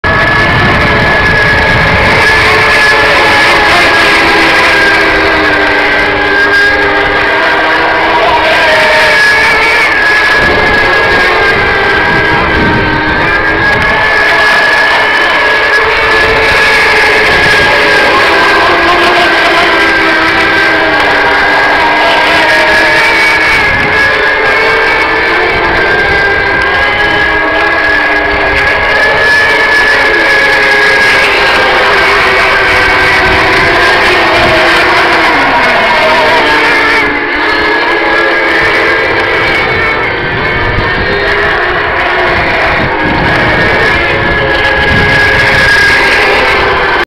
Bridlington Multirace Action
This clip depicts a race that demonstrates superb driving of very fast boats. 9 May 2010 and 10 out of 10 to the excellent driving skills displayed today.